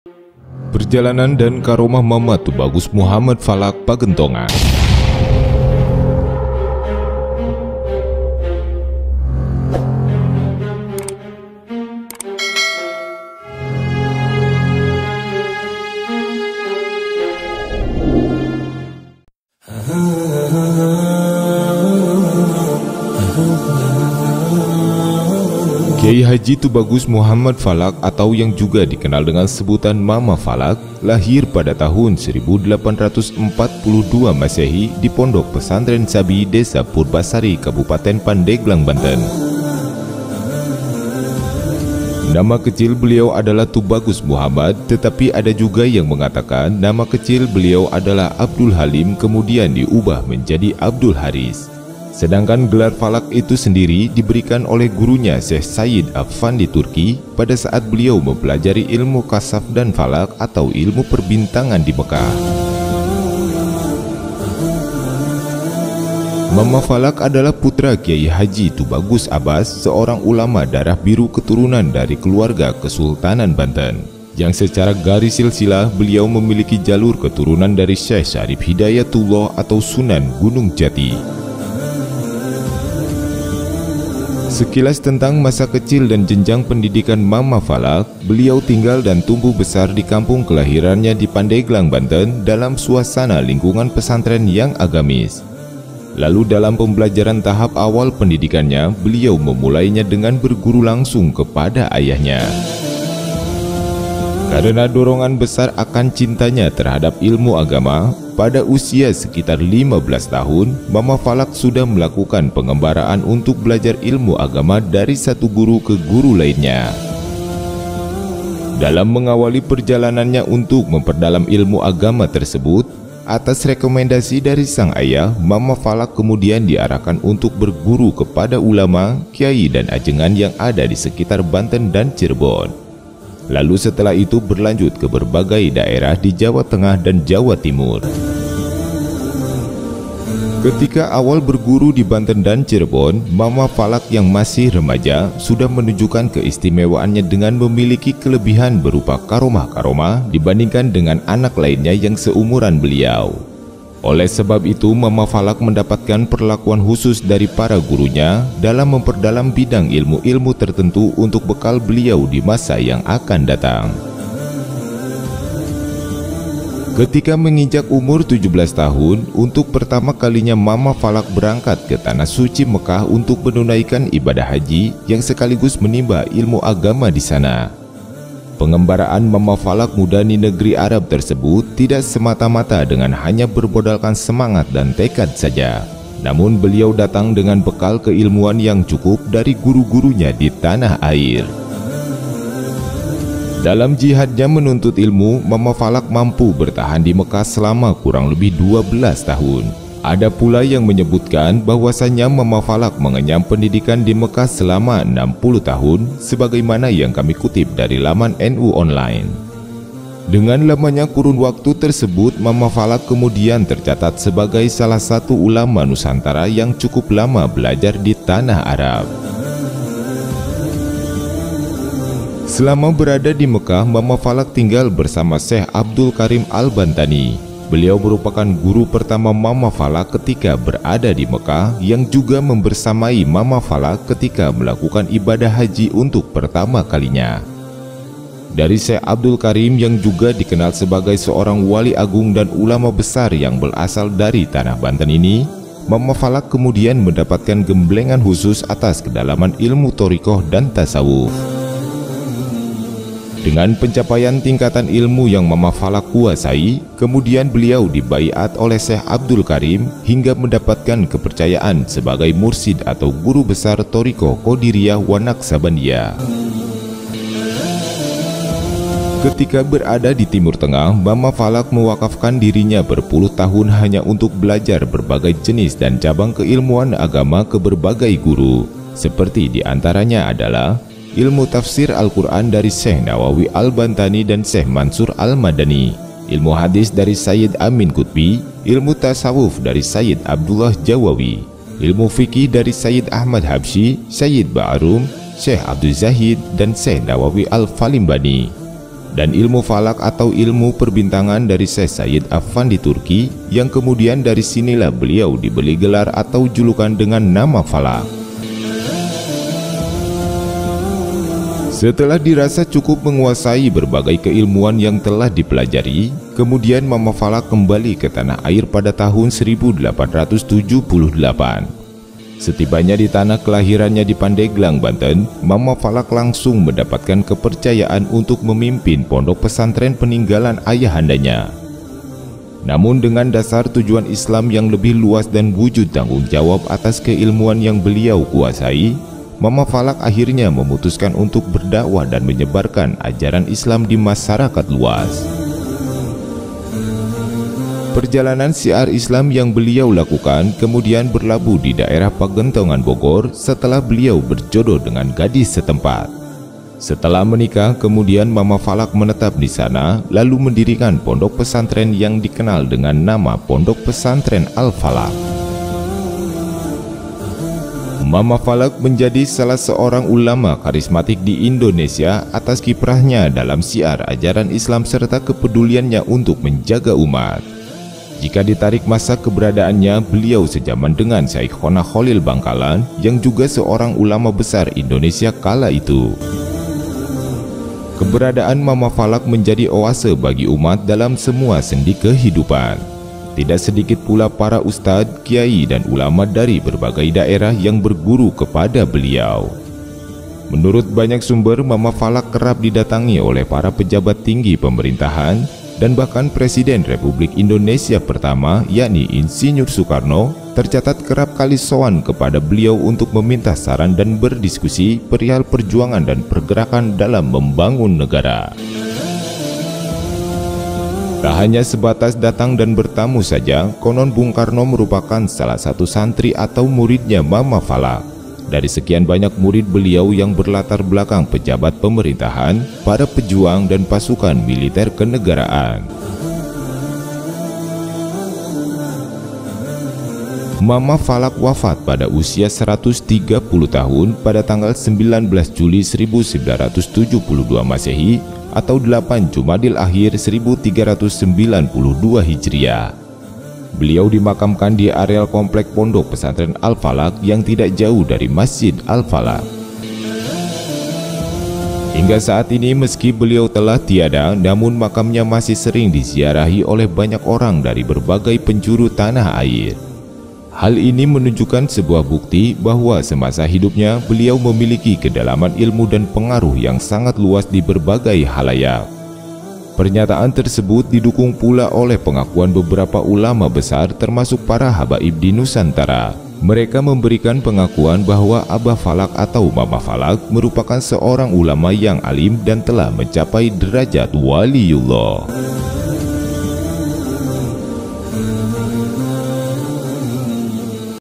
WALIYULLAH AHLI ILMU PERBINTANGAN - Kisah Singkat Tentang KH. Tubagus FALAK // MAMA FALAK
Selama berada di mekkah mama falak tinggal bersama Syekh Abdul Karim albantani, beliau merupakan guru pertama mama falak ketika berada dimekah’ yang juga membersamai mama falak, ketika melakukan ibadah haji untuk pertama kalinya. Dari Syekh Abdul Karim, yang juga dikenal sebagai seorang wali agung dan ulama besar yang berasal tanah banten ini, mama falak kemudian mendapatkan gemblengan khusus’ atas kedalaman ilmu thorekoh dan tasawuf. Dengan pencapaian tingkatan ilmu yang mama Falak kuasai, kemudian beliau dibai’at  oleh syekh abdul karim, hingga mendapatkan kepercayaan sebagai mursyid atau guru besar, Thoriqoh Qodiriyah wa Naqsyabandiyah.